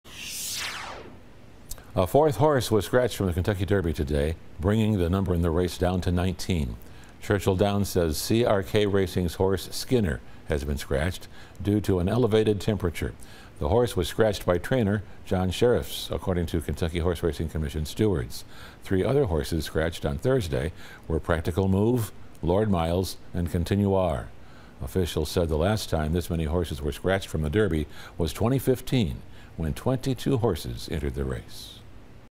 Fourth horse scratched from derby, 19 remain
Officials say the last time this many horses were scratched from the derby was 2015 when 22 horses entered the race.